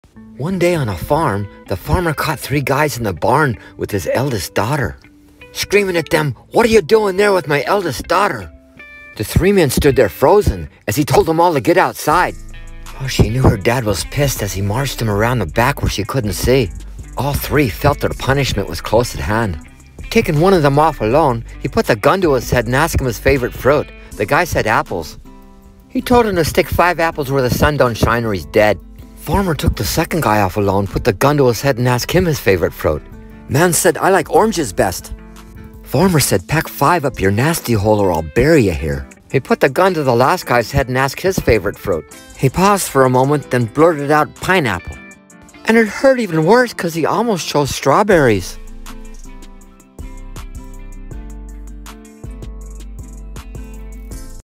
FRUITVILLE
Really bad for three guys that got caught, playing ￼￼with the farmers daughter, it literally didn’t end well